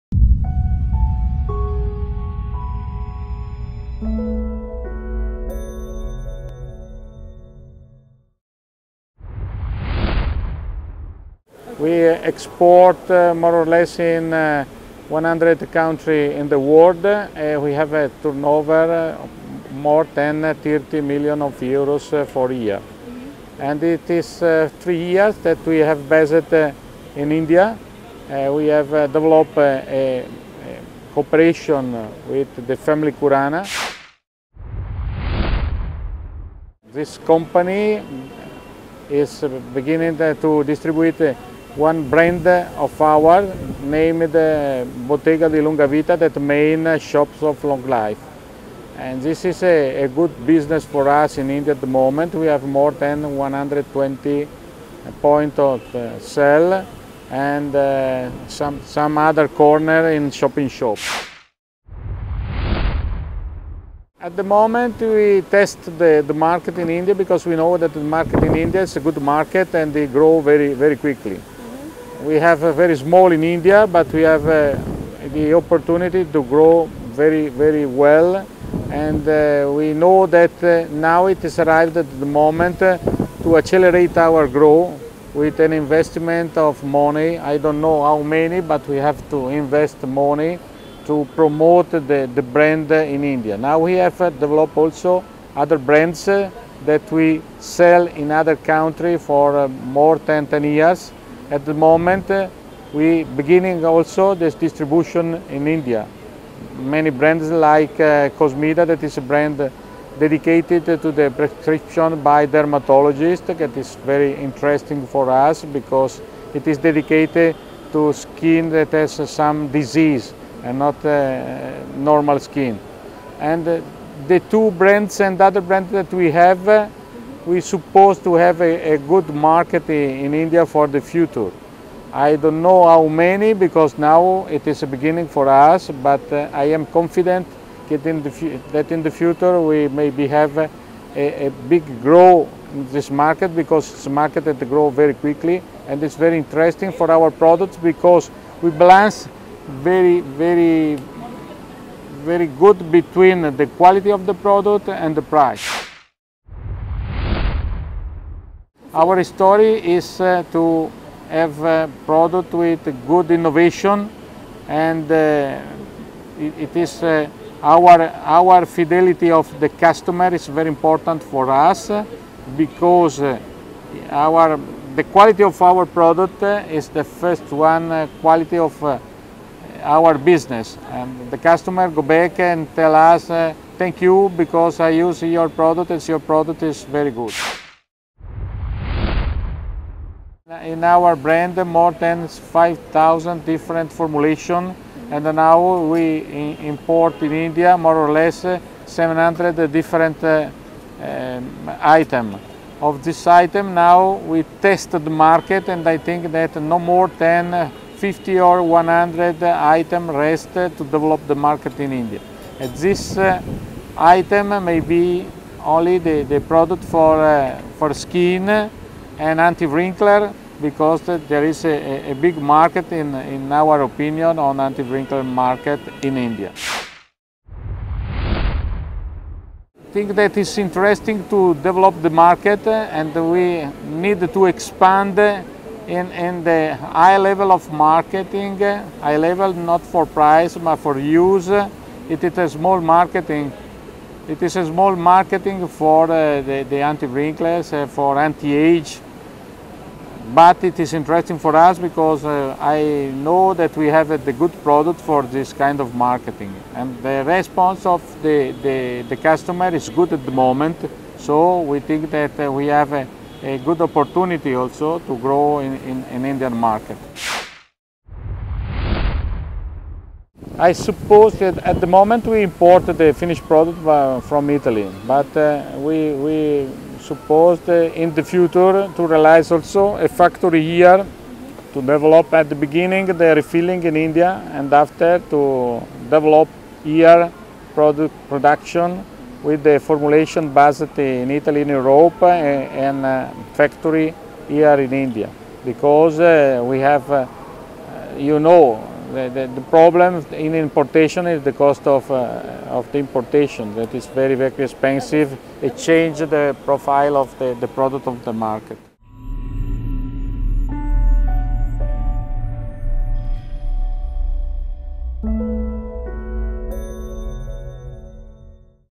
Betting high on Indian beauty market; Plans to open factory in future: Bottega President
Italian skincare brand, Bottega di LungaVita has  entered into Indian beauty market in 2013. The international brand is holding a reputed position in the European markets with 20 per cent market share in skin care segment. In span of two years, the brand has established a very strongest presence in the Indian market as well, especially in northern region. Gradually moving towards Western region in cities like Goa, Mumbai etc, the brand have risen to approx 130 points of sale, with 6 flagship stores - 2 in Delhi , and 1 each in Mizoram , Chandigarh , Ludhiana and Goa. In a recent visit to India, WI Bureau shared a candid moment with ALBERTO DONATI, Brand Owner & President, Bottega Di Lungavita, Officina Farmaceutica Italiana (OFI), who is laying high hopes on Indian beauty market.